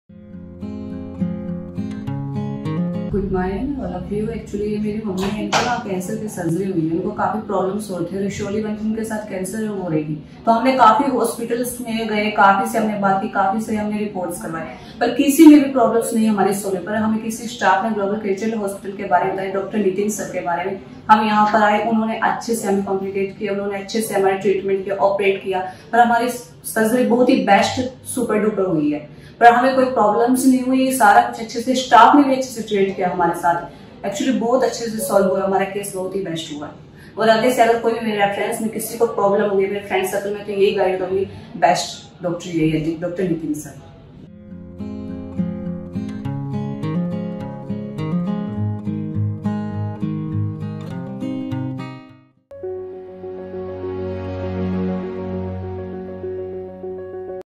𝐏𝐚𝐭𝐢𝐞𝐧𝐭 𝐓𝐞𝐬𝐭𝐢𝐦𝐨𝐧𝐢𝐚𝐥 | 𝐃𝐫 𝐍𝐢𝐭𝐢𝐧 𝐁𝐚𝐧𝐬𝐚𝐥 | 𝐆𝐥𝐨𝐛𝐚𝐥 𝐂𝐫𝐚𝐝𝐥𝐞 𝐇𝐨𝐬𝐩𝐢𝐭𝐚𝐥
𝐏𝐚𝐭𝐢𝐞𝐧𝐭 𝐓𝐞𝐬𝐭𝐢𝐦𝐨𝐧𝐢𝐚𝐥 𝐨𝐧 𝐋𝐚𝐩𝐚𝐫𝐨𝐬𝐜𝐨𝐩𝐢𝐜 𝐑𝐞𝐝𝐢𝐜𝐚𝐥 𝐡𝐲𝐬𝐭𝐞𝐫𝐞𝐜𝐭𝐨𝐦𝐲 𝐟𝐨𝐫 𝐜𝐚𝐫𝐜𝐢𝐧𝐨𝐦𝐚 𝐞𝐧𝐝𝐨𝐦𝐞𝐭𝐫𝐢𝐮𝐦

𝐆𝐨𝐛𝐚𝐥 𝐂𝐫𝐚𝐝𝐥𝐞 𝐇𝐨𝐬𝐩𝐢𝐭𝐚𝐥
𝐀𝐳𝐚𝐝 𝐧𝐚𝐠𝐚𝐫, 𝐇𝐚𝐧𝐬𝐢 𝐑𝐨𝐚𝐝, 𝐒𝐚𝐧𝐭 𝐍𝐚𝐠𝐚𝐫, 𝐀𝐝𝐣𝐨𝐢𝐧𝐢𝐧𝐠 𝐒𝐚𝐧𝐭 𝐍𝐢𝐫𝐚𝐧𝐤𝐚𝐫𝐢 𝐁𝐡𝐚𝐰𝐚𝐧, 𝐤𝐚𝐫𝐧𝐚𝐥, 𝐇𝐚𝐫𝐲𝐚𝐧𝐚 𝟏𝟑𝟐𝟎𝟎𝟏, 𝐈𝐧𝐝𝐢𝐚